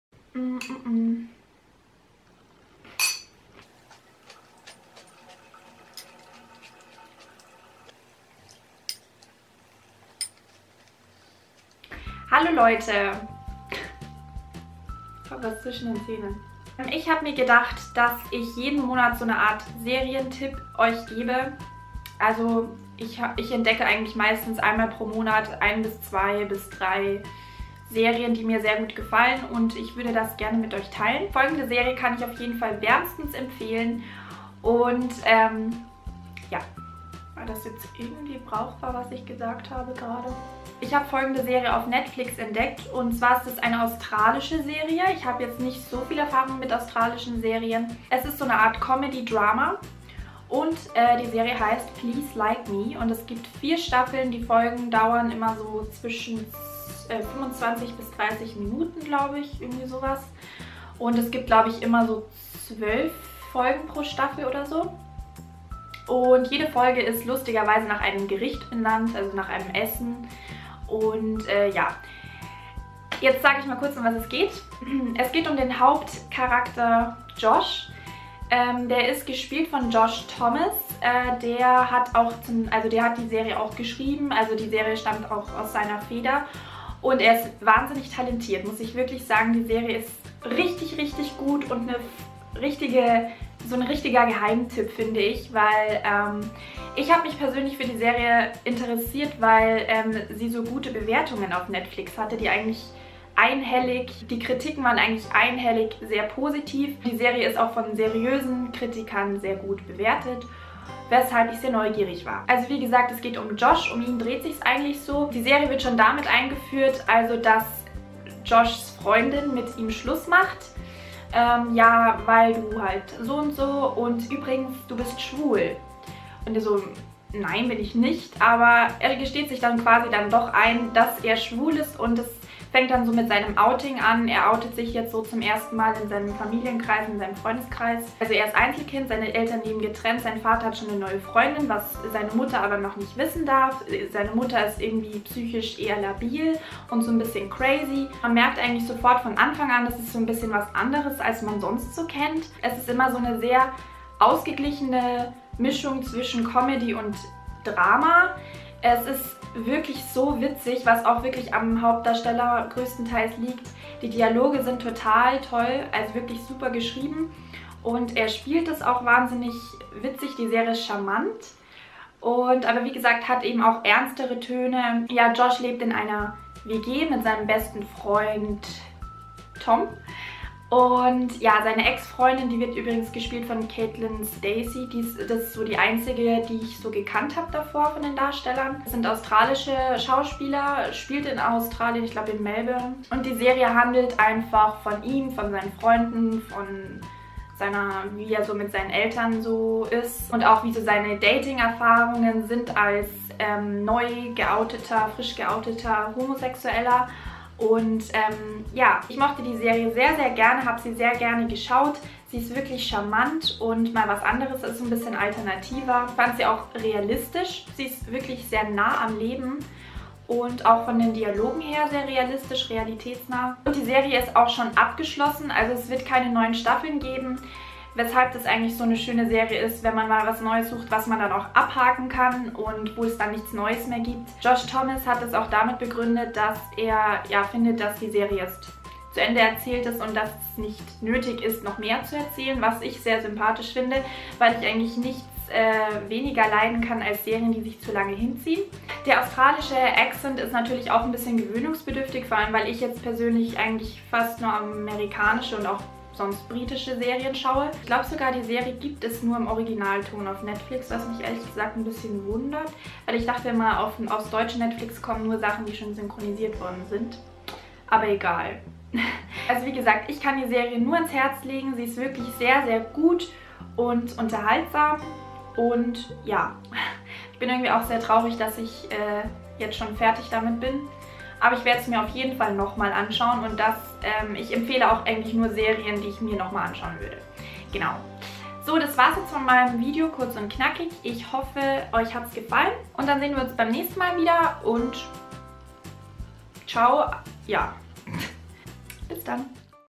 PLEASE LIKE ME | Mein Serientipp im April
PLEASE LIKE ME | Mein Serientipp im April | Theresa Katharina

Meine Serienempfehlung des Monats!
"PLEASE LIKE ME" created by Josh Thomas
ABC Network, Produktionsland: Australien
Erstausstrahlung: 23. Februar 2013
Genre: Comedy, Drama
4 Staffeln, 32 Episoden
mit Josh Thomas, Debra Lawrance, David Roberts, Thomas John Ward, Wade Briggs, Caitlin Stasey...